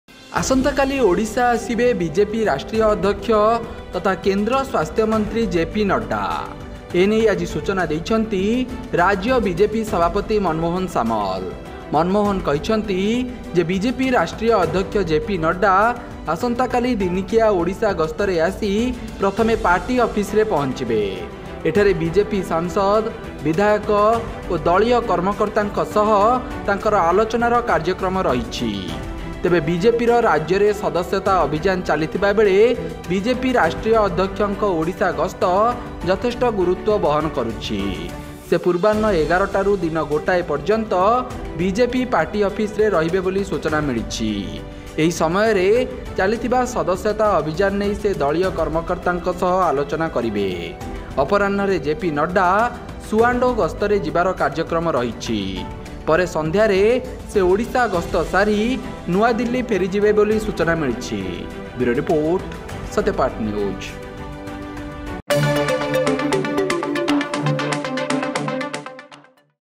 କାଲି Odisha ଆସିବେ JP Nadda , ଜାଣନ୍ତୁ କଣ ରହିଛି କାର୍ଯ୍ୟକ୍ରମ || Satyapatha News
କାଲି ଓଡିଶା ଆସିବେ ନଡ୍ଡା, ଜାଣନ୍ତୁ କଣ ରହିଛି କାର୍ଯ୍ୟକ୍ରମ

Click on the playlist 'Satyapatha With Satyaprakash' & watch all the editorial episodes.